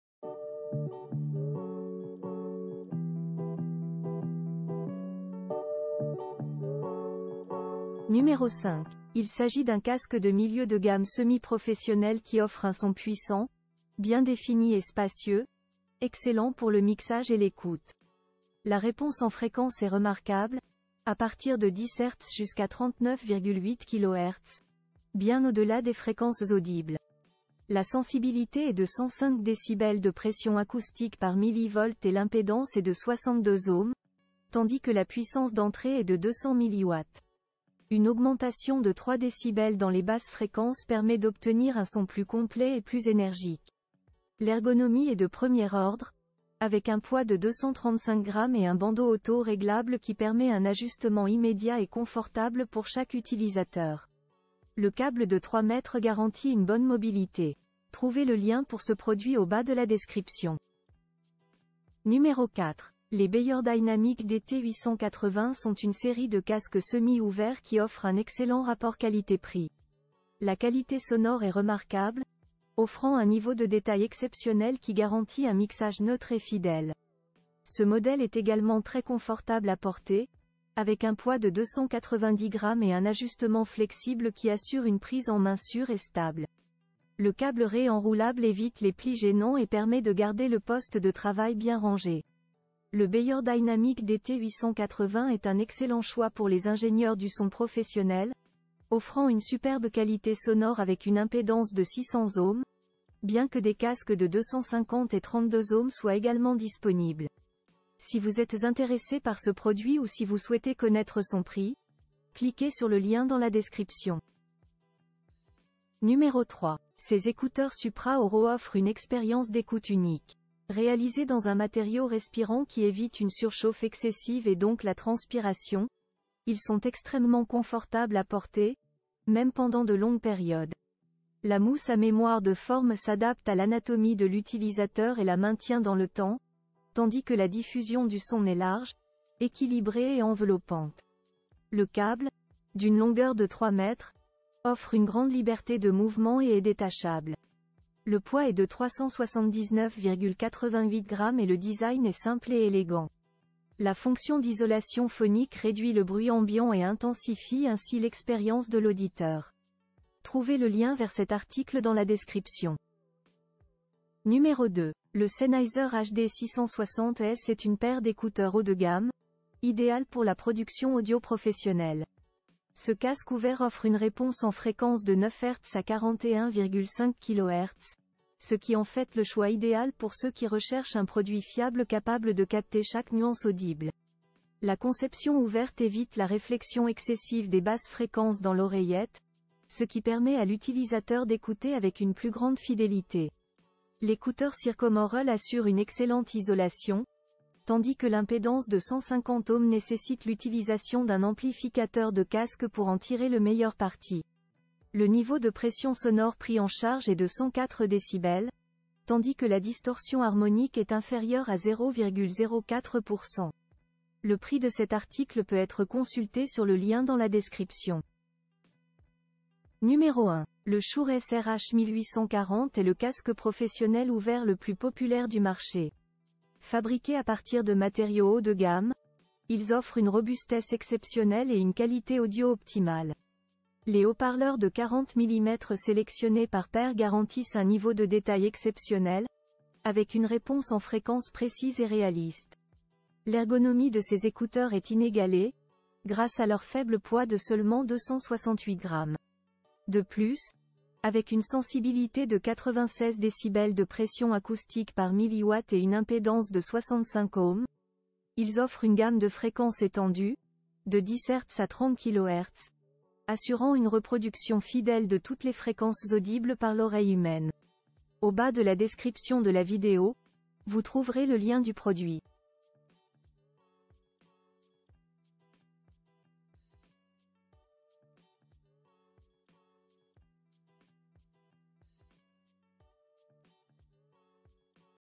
Les 5 meilleurs casques ouverts
1) Shure - SRH1840 BK.
2) Sennheiser HD 660 S Casqu... »
3) Philips Audio - Philips F... »
4) Beyerdynamic Casque Hi-Fi... »
5) AKG K712PROCasque de réfé... »

✋ Voici les 5 meilleurs casques ouverts, disponibles sur le marché.

00:00 - Introduction
00:08 - 5) AKG K712PROCasque de référence haut de gamme circum-aural ou...
01:01 - 4) Beyerdynamic Casque Hi-Fi DT 880 Edition 600 ohms.
01:55 - 3) Philips Audio - Philips Fidelio X3/00 Casque Circum-aural Fi...
02:44 - 2) Sennheiser HD 660 S Casque Dynamique Ouvert.
03:39 - 1) Shure - SRH1840 BK.